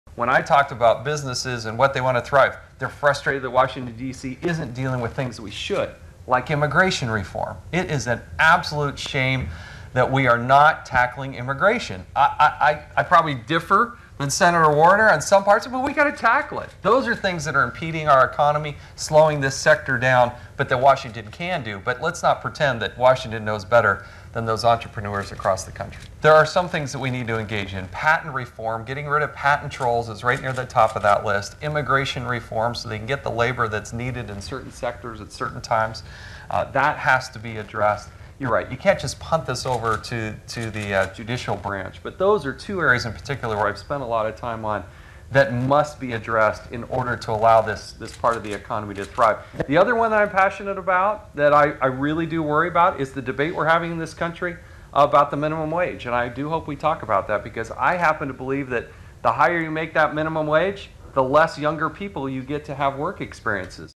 Jason Chaffetz at Generation Next event
Rep. Jason Chaffetz's comments at the POLITICO Generation Next event.